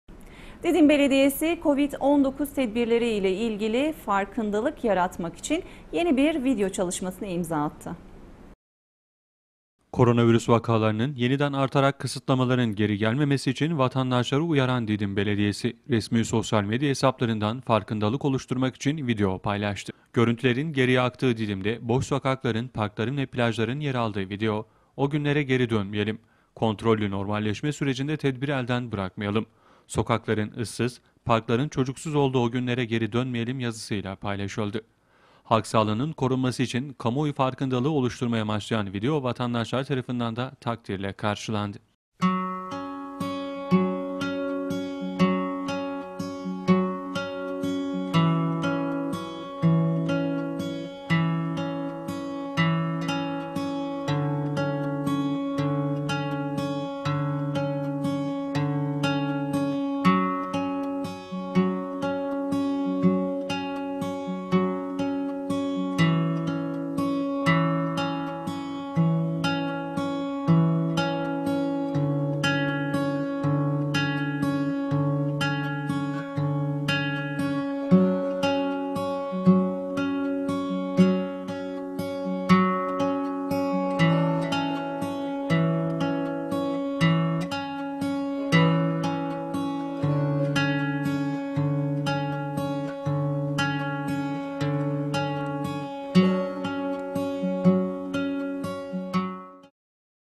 DİDİM BELEDİYESİ UYARDI, O GÜNLERE GERİ DÖNMEYELİM
DİDİM BELEDİYESİ COVİD-19 TEDBİRLERİ İLE İLGİLİ FARKINDALIK YARATMAK İÇİN YENİ BİR VİDEO ÇALIŞMASINA İMZA ATTI